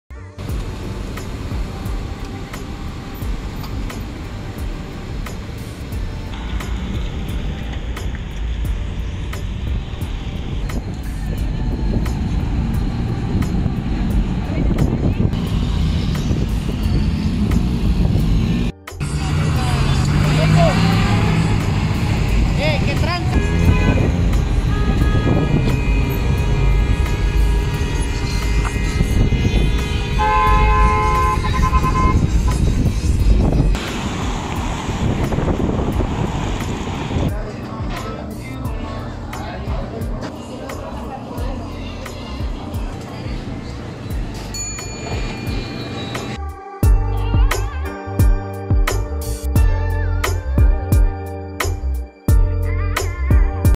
En Tijuana retan al Covid19 y ante semáforo en rojo en BajaCalifornia.
Tijuanenses retan al Covid19 a pesar de que Tijuana registró 136 casos nuevos de Coronavirus en las últimas 2 semanas y 15 muertos en las últimas 24 horas.